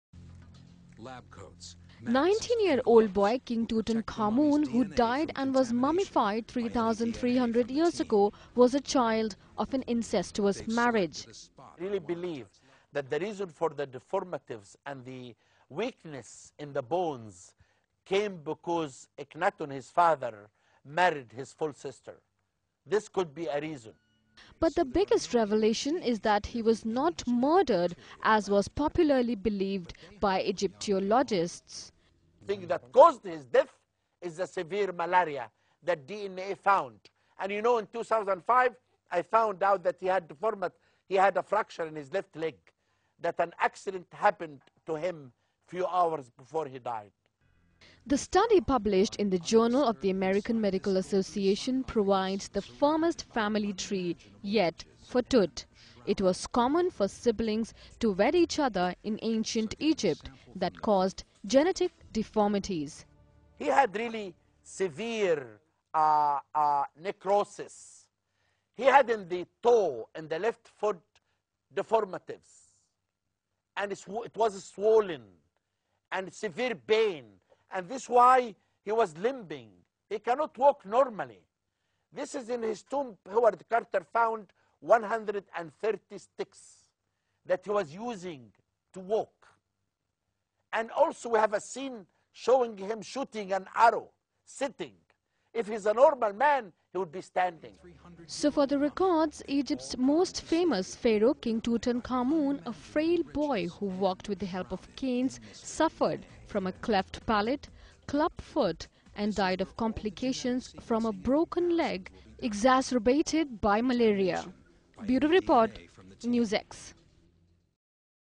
Malaria Is a Likely Killer in King Tuts Post-Mortem
King Tutankhamen, the boy pharaoh, was frail and lame and suffered multiple disorders when he died at age 19 about 1324 B.C., but scientists have now determined the most likely agents of death: a severe bout of malaria combined with a degenerative bone condition.